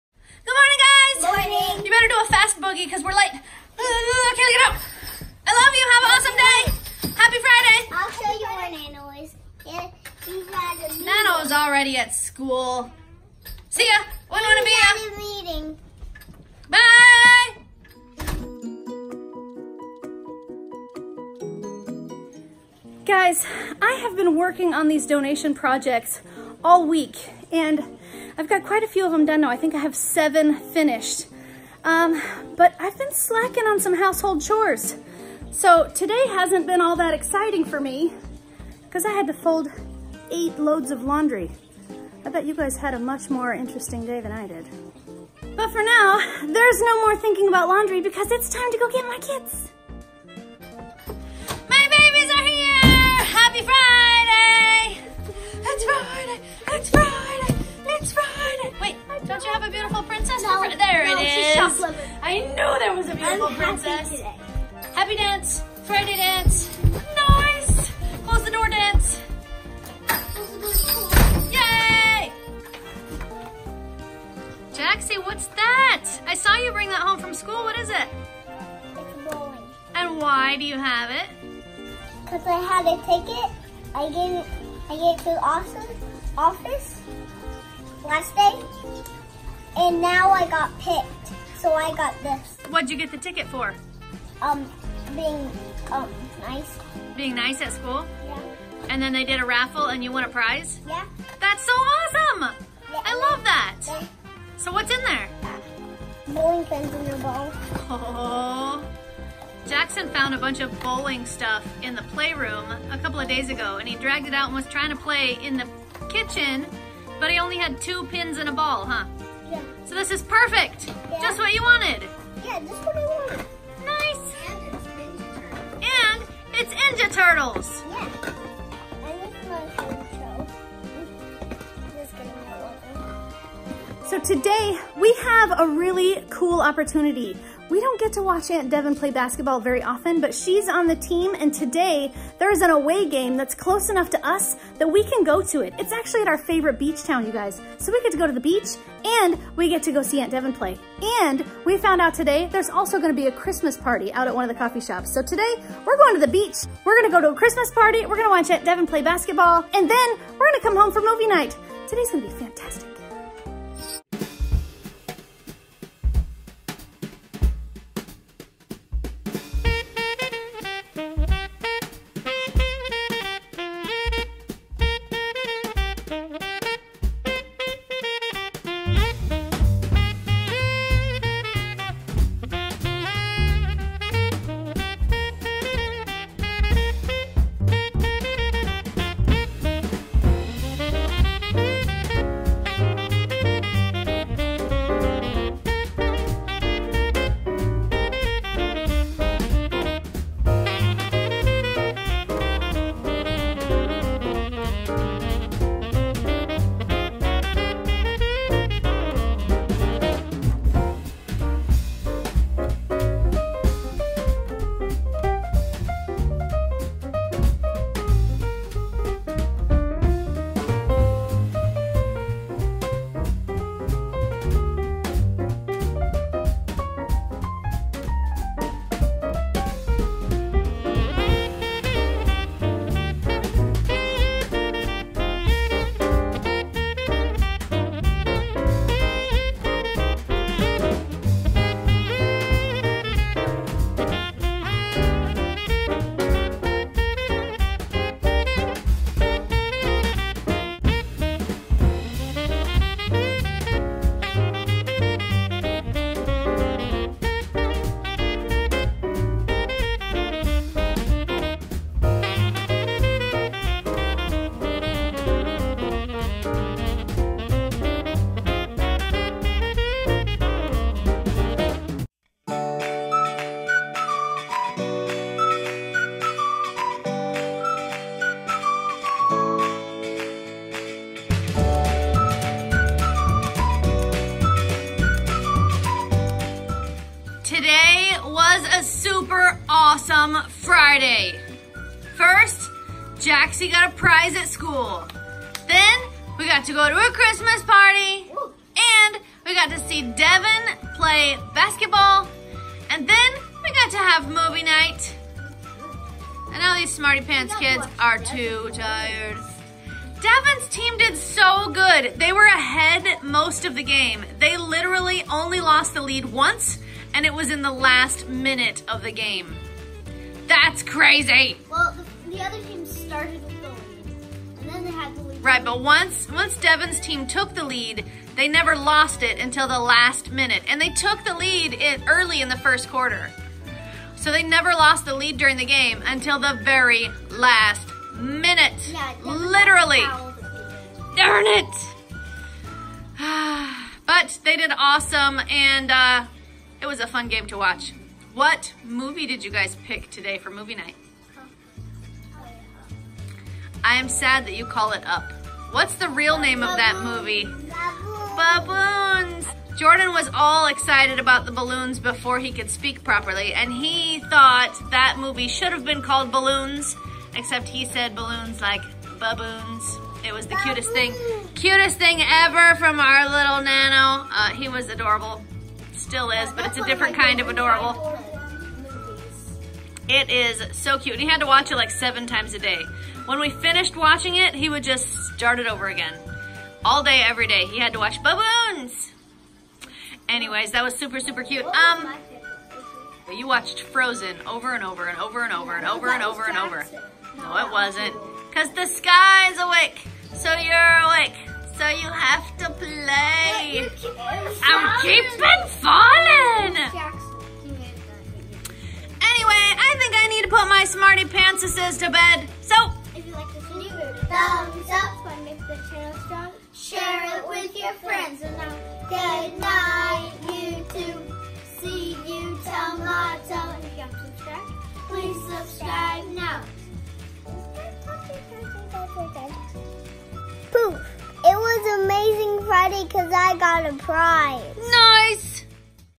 A Good Behavior Prize
The Smartypants Kids’ school gives out “behavior points” and awards kids with fun raffles and prizes for doing their best.  Today A Good Behavior Prize came home from school!